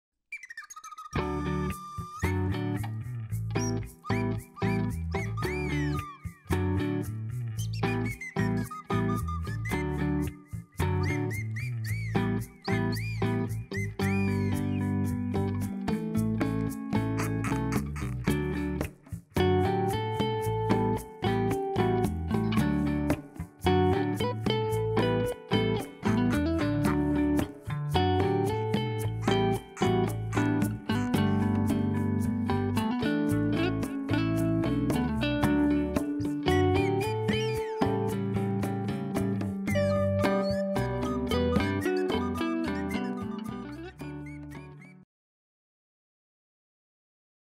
Grey Parrot playing with crocodile funny
The Grey Parrot is still a baby and loves to play, loves to gnaw everything. Now the Grey Parrot is plays with the crocodile funny)